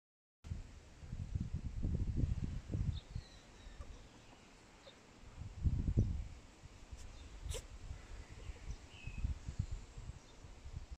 Oh so vicious ferret attacks garden rabbit
Here you'll see the wild yet domestic Garden Goose, species; ferret attack and a wild garden rabbit 😂